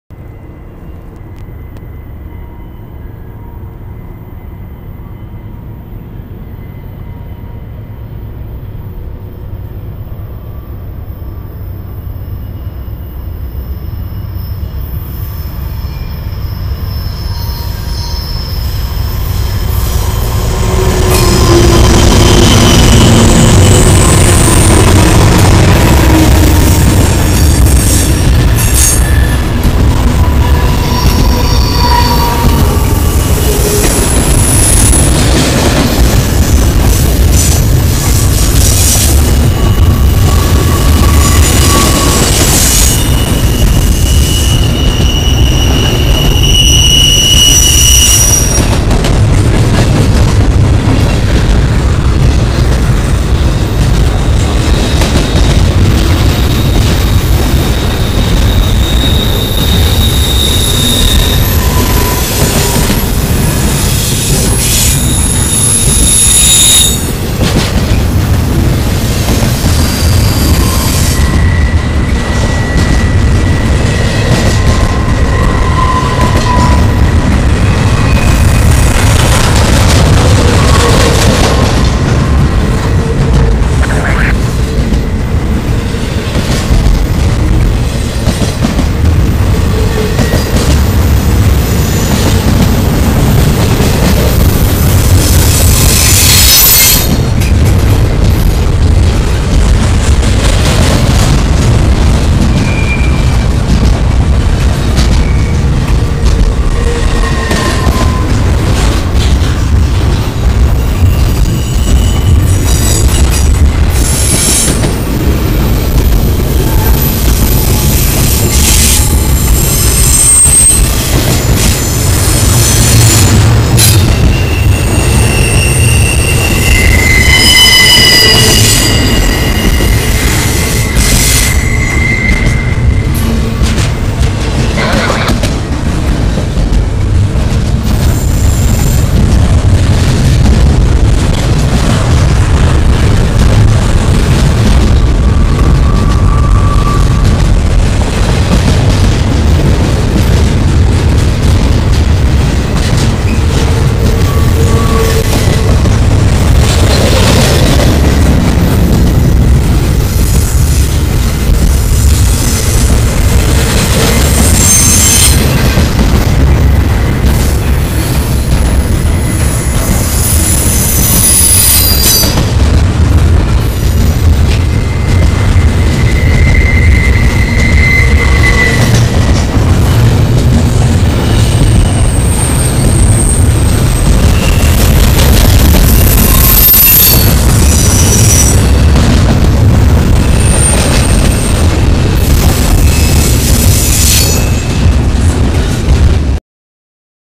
Railfanning Kenova, West Virginia. NS 9414 Leads Train 233 West.
NS 9414 is seen at Kenova, West Virginia as it leads intermodal train 233 west towards Portsmouth and Columbus. This as it approaches the former Norfolk & Western bridge spanning the entire town of Kenova, CSX Transportation's Kanawha Subdivision, and the Ohio River. This is on Norfolk Southern's Kenova District that is apart of Norfolk Southern's "Pocahontas Division."